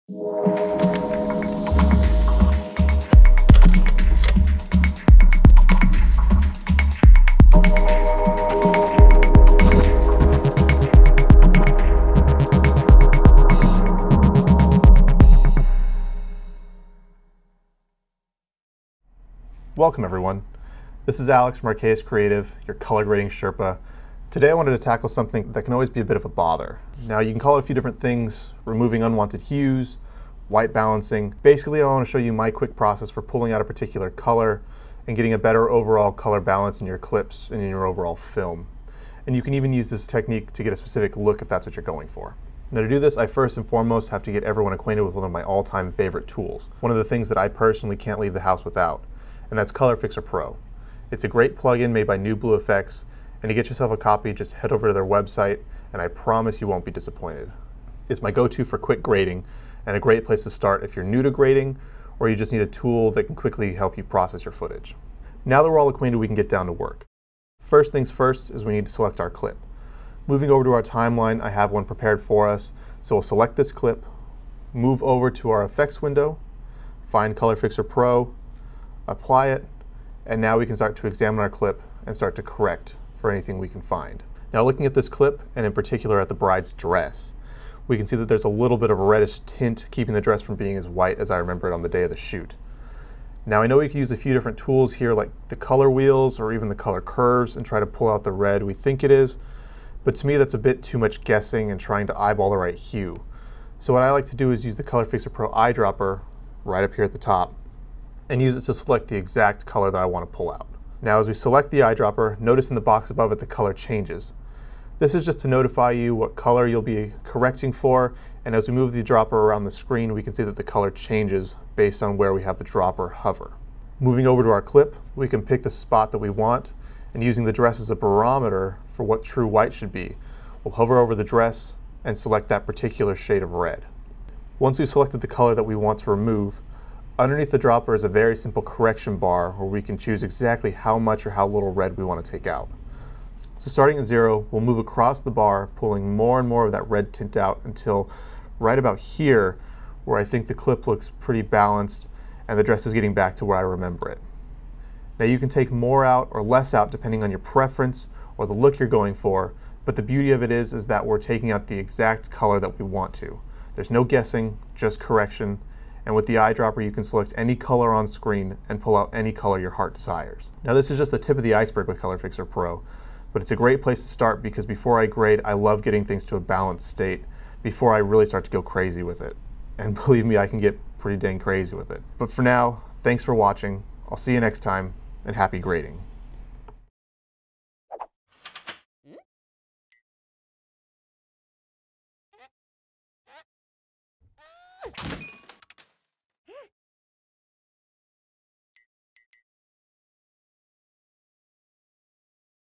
Color Fixer Pro - Adobe Premiere Pro CC 💻
Alex from Archaius Creative a Post House for Wedding and Commercial work walks us through an introduction to Color Correction using Color Fixer Pro in Premiere Pro CC.